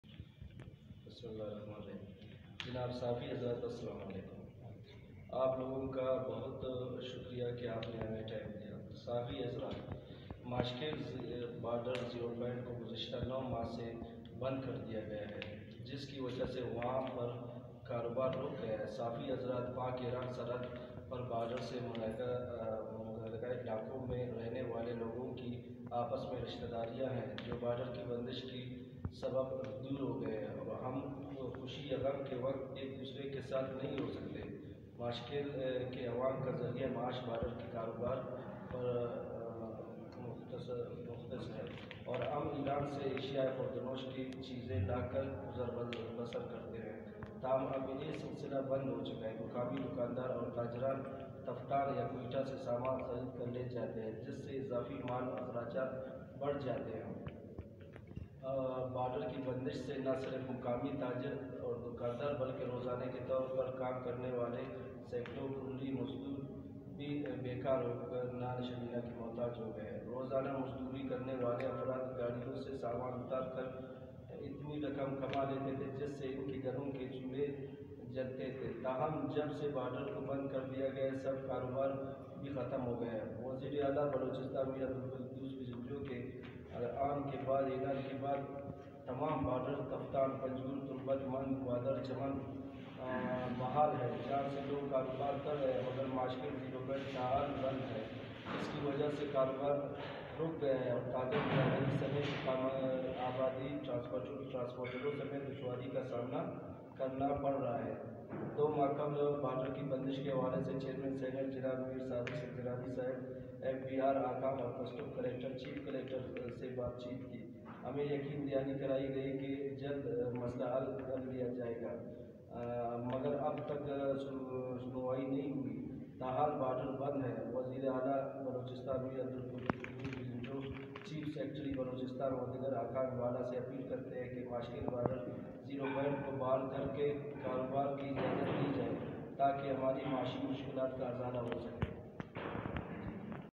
ماشکیل ٹرانسپورٹر کی مشترکہ پریس کانفرنس
ماشکیل بارڈرزیروپوائنٹ کوگزشتہ9ماہ سے بندکردیاگیا ہے جس کی وجہ سے وہاں پرکاروباررُک گیا ہے۔  پاک ایران سرحد پربارڈرسے ملحقہ علاقوں میں رہنے والے لوگوں کی آپس میں رشتہ داریاں ہیں جوبارڈرکی بندش کے سبب دورہوگئے ہیں اورہم خوشی یاغم کے وقت ایک دوسرے کے ساتھ نہیں ہوسکتے۔ ماشکیل کے عوام کاذریعہ معاش بارڈرکے کاروبارپرمنحصرہے اورہم ایران سے اشیاء خوردونوش کی چیزیں لاکرگزربسرکرتے تھے تاہم اب یہ سلسلہ بندہوچکاہے۔
  مقامی دکانداراورتاجران تفتان یاکوئٹہ سے سامان خریدکرلے جاتے ہیں جس سے اضافی مالی اخراجات بڑھ گئے ہیں۔ بارڈرکی بند ش سے نہ صرف مقامی تاجراوردکانداربلکہ روزانہ کے طورپرکام کرنے والے سینکڑوں قلی اورمزدوربھی بے کارہوکر نان شبینہ کے محتاج ہوگئے ہیں۔ روزانہ مزدوری کرنے والے افرادگاڑیوں سے سامان اتارکراتنی رقم کمالیتے تھے جس سے ان کے گھروں کے چولہے جلتے تھے تاہم جب سے بارڈرکوبندکردیاگیا ہے سب کاروزگاربھی ختم ہوگیا
 وزیراعلیٰ بلوچستان میرعبدالقدوس بزنجوکے العان کے بعد تمام بارڈرتفتا، پنجگور، تربت، مند، گوادراورچمن بحال ہیں جہاں سے لوگ کاروبارکررہے ہیں مگرماشکیل زیروپوائنٹ تاحال بندہے جس کی وجہ سے کاروباررک گیا ہے اورتاجربرادری سمیت مقامی آبادی کو دشواری کاسامناکرناپڑرہا ہے۔

 2ماہ قبل بارڈرکی بندش کے حوالے سے چیئرمین سینیٹ میرصادق سنجرانی، ایف بی آر حکام اورکسٹم کلیکٹرسے بات چیت کی ہمیں یقینی دہانی کرائی کی گئی کہ جلدمسئلہ حل کرلیاجائے گا مگراب تک شنوائی نہیں ہوئی اورتاحال بارڈربندہے۔